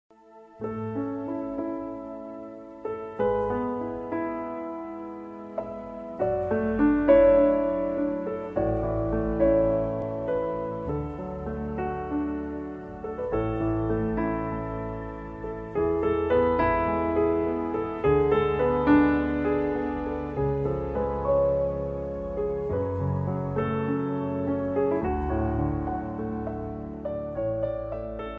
FM ZhuYe Motorized Adjustable Venetian Blinds, Aluminum Blackout Electric Horizontal Blinds FM ZhuYe
Glides up and down on guiding cables with a touch of the remote control. One remote control can control multiple sets to meet the needs of home.